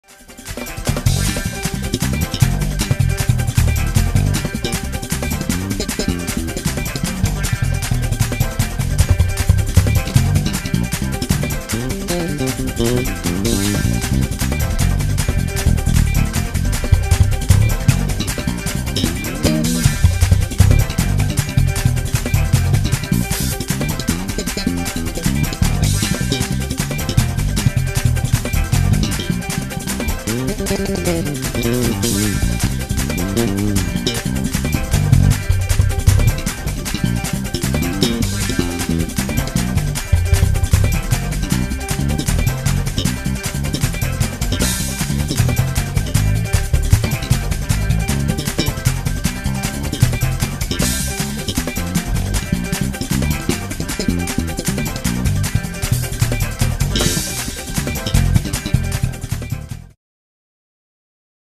Sunday Night Praise Break Derick Mcelveen Jr. X 6 Strangs Play Along
❗️Sunday Night Praise Break❗️

Played along with my dude again Derick Mcelveen Jr. on drums...